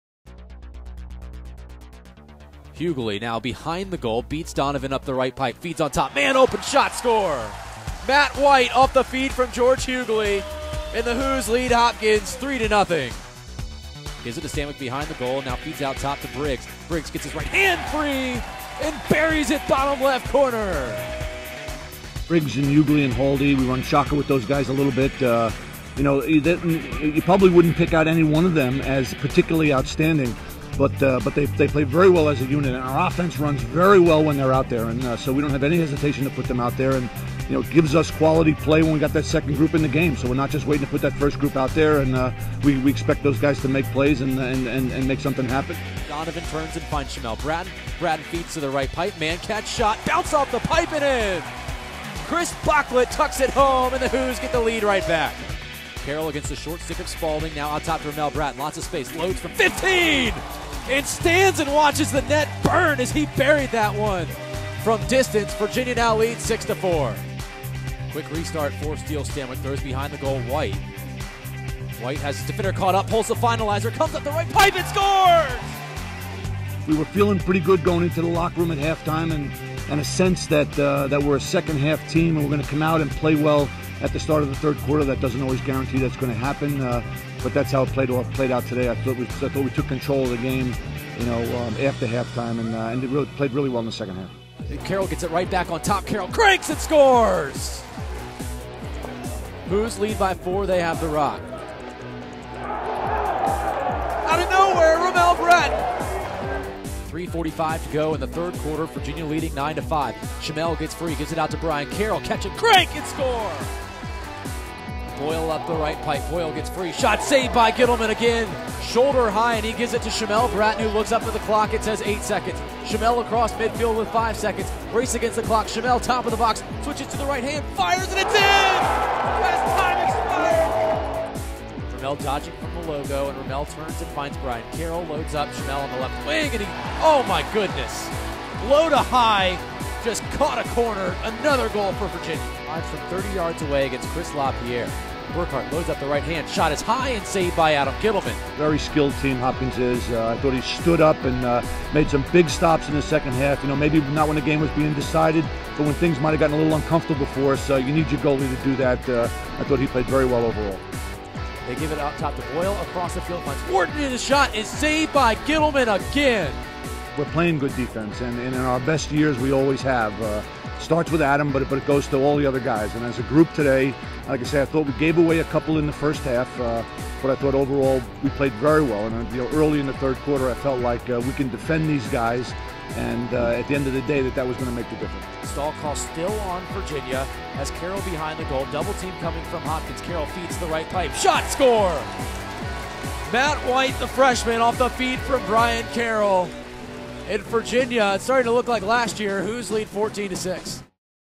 #1 Virginia Men's Lacrosse Downs Johns Hopkins
#1 Virginia defeats #12 Johns Hopkins 15-6 at Klockner Stadium.